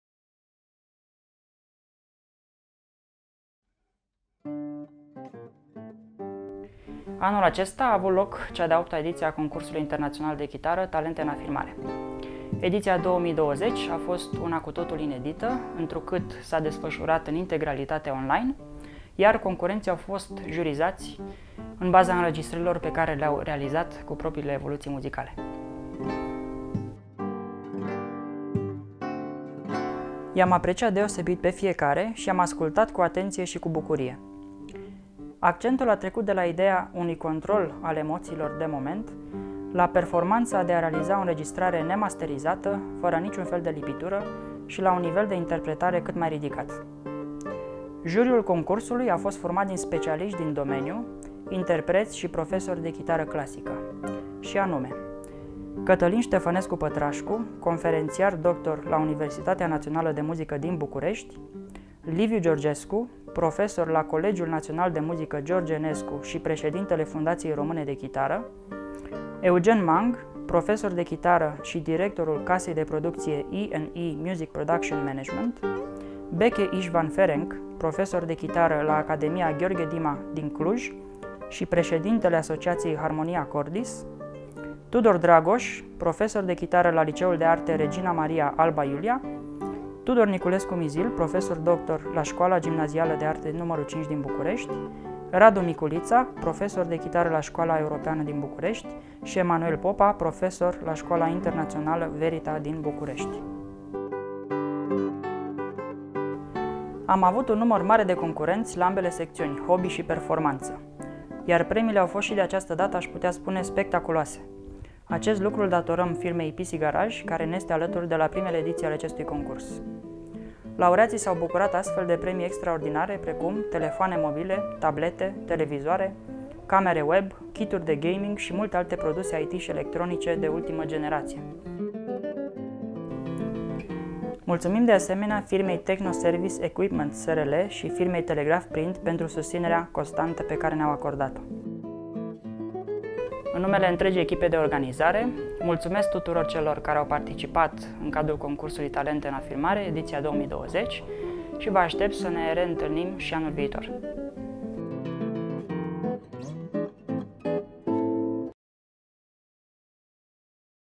Concursul international de chitara clasica "TALENTE IN AFIRMARE", 2020, editia a VIII-a, ONLINE
Concursul international de chitara clasica "TALENTE IN AFIRMARE", 2020, editia a VIII-a, ONLINE. Premiile au fost oferite de PC GARAGE
The "Upcoming Guitar Talents" International Competition, 2020, 8th Edition, ONLINE. The prizes were awarded by PC GARAGE